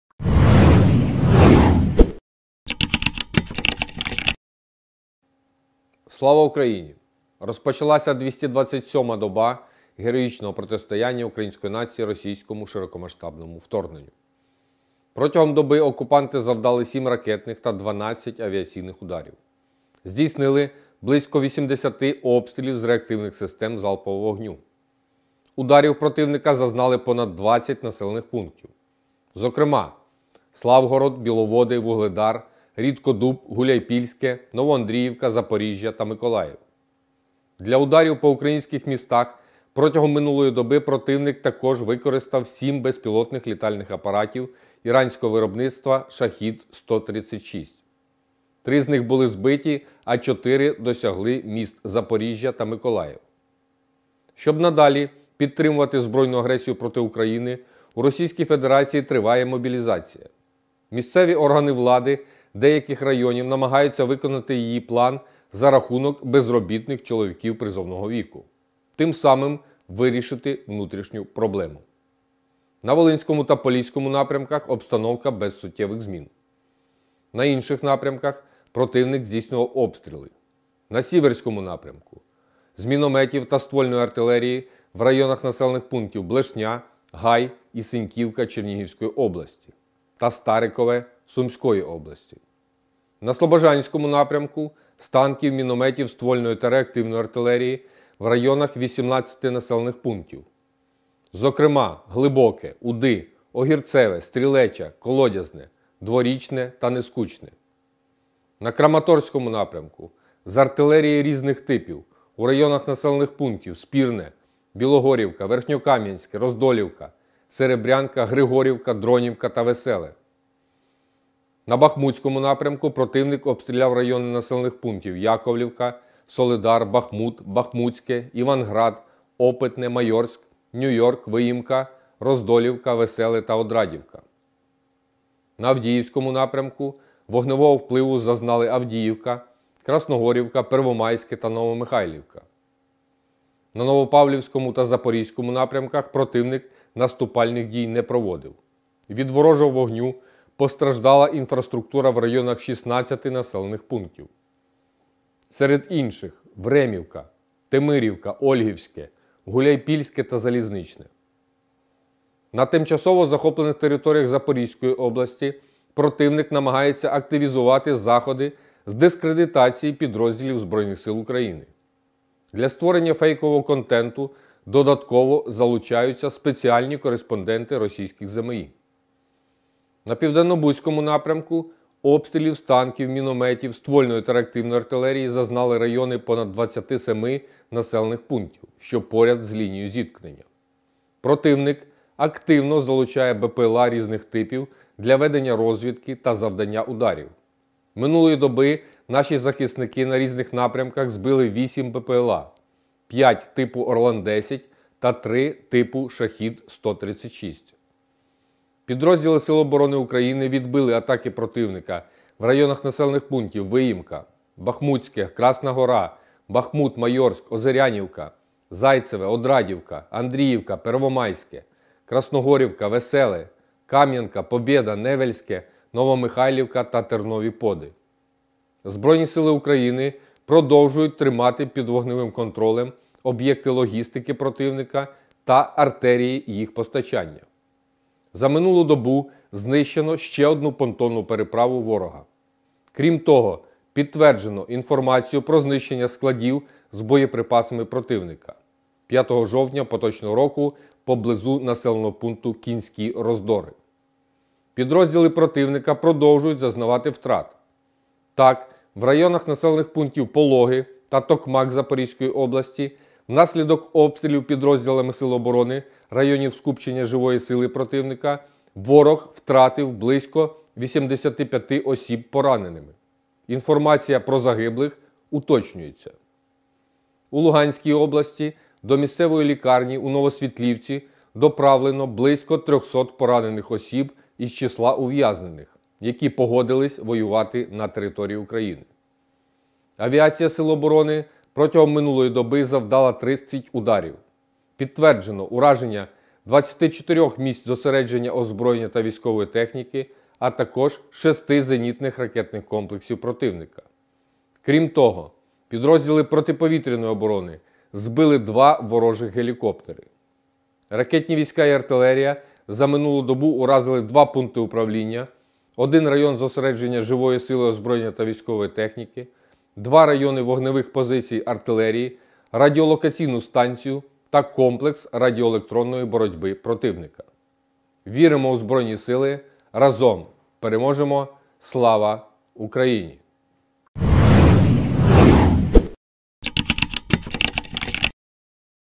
Оперативна інформація станом на 06:00 08.10.2022 щодо російського вторгнення
Слава Україні! Розпочалася двісті двадцять сьома доба героїчного протистояння Української нації російському широкомасштабному вторгненню.

Протягом доби окупанти завдали 7 ракетних та 12 авіаційних ударів, здійснили близько 80 обстрілів з реактивних систем залпового вогню.

Ударів противника зазнали понад 20 населених пунктів. Зокрема, Славгород, Біловоди, Вугледар, Рідкодуб, Гуляйпільське, Новоандріївка, Запоріжжя та Миколаїв.

Для ударів по українських містах протягом минулої доби противник також використав сім безпілотних літальних апаратів іранського виробництва “Shahed -136”. Три з них були збиті, а чотири досягли міст Запоріжжя та Миколаїв.

Щоб надалі підтримувати збройну агресію проти України, у російській федерації триває мобілізація. Місцеві органи влади деяких районів намагаються виконати її план за рахунок безробітних чоловіків призовного віку, тим самим вирішити внутрішню проблему.

На інших напрямках противник здійснював обстріли:

на Сіверському напрямку – з мінометів та ствольної артилерії в районах населених пунктів Блешня, Гай і Сеньківка Чернігівської області та Старикове Сумської області;

на Слобожанському напрямку – з танків, мінометів, ствольної та реактивної артилерії, в районах 18 населених пунктів, в тому числі Глибоке, Уди, Огірцеве, Стрілеча, Колодязне, Дворічне, Нескучне;

на Краматорському напрямку – з артилерії різних типів, у районах населених пунктів Спірне, Білогорівка, Верхньокам’янське, Роздолівка, Серебрянка, Григорівка, Дронівка, Веселе;

на Бахмутському напрямку противник обстріляв райони населених пунктів Яковлівка, Соледар, Бахмут, Бахмутське, Іванград, Опитне, Майорськ, Нью-Йорк, Виїмка, Роздолівка, Веселе та Одрадівка;

на Авдіївському напрямку – вогневого впливу зазнали Авдіївка, Красногорівка, Первомайське та Новомихайлівка.

На Новопавлівському та Запорізькому напрямках противник наступальних дій не проводив. Від ворожого вогню постраждала інфраструктура в районах 16 населених пунктів. Серед інших - Времівка, Темирівка, Ольгівське, Гуляйпільське та Залізничне.

На тимчасово захоплених територіях Запорізької області противник намагається активізувати заходи з дискредитації підрозділів Збройних Сил України. Для створення фейкового контенту додатково залучаються спеціальні кореспонденти російських ЗМІ.

На Південнобузькому напрямку обстрілів з танків, мінометів, ствольної та реактивної артилерії зазнали райони понад 27 населених пунктів, що поряд з лінією зіткнення.

Противник активно залучає БпЛА різних типів для ведення розвідки та завдання ударів. Минулої доби наші захисники на різних напрямках збили вісім БпЛА: п’ять типу “Орлан-10” та три типу “Shahed -136”.

Підрозділи Сил оборони України відбили атаки противника в районах населених пунктів Виїмка, Бахмутське, Красна Гора, Бахмут, Майорськ, Озерянівка, Зайцеве, Одрадівка, Андріївка, Первомайське, Красногорівка, Веселе, Кам’янка, Побєда, Невельське, Новомихайлівка та Тернові Поди.

Збройні Сили України продовжують тримати під вогневим контролем об’єкти логістики противника та артерії їх постачання. За минулу добу знищено ще одну понтонну переправу противника. Крім того, підтверджено інформацію про знищення складів з боєприпасами ворога, 5 жовтня поточного року, поблизу населеного пункту Кінські Роздори.

Підрозділи противника продовжують зазнавати втрат. Так, в районах населених пунктів Пологи та Токмак Запорізької області, внаслідок обстрілів підрозділами Сил оборони районів скупчення живої сили противника, ворог втратив близько 85 осіб пораненими. Інформація про загиблих уточнюється.

У Луганській області до місцевої лікарні у Новосвітлівці доправлено близько трьохсот поранених осіб із числа ув’язнених, які погодились воювати на території Україні.

Авіація Сил оборони протягом минулої доби завдала 30 ударів. Підтверджено ураження 24 місць зосередження озброєння та військової техніки, а також 6 зенітних ракетних комплексів противника. Крім того, підрозділи протиповітряної оборони збили два ворожих гелікоптери.

Ракетні війська і артилерія за минулу добу уразили два пункти управління, один район зосередження живої сили, озброєння та військової техніки, два райони вогневих позицій артилерії, радіолокаційну станцію та комплекс радіоелектронної боротьби противника.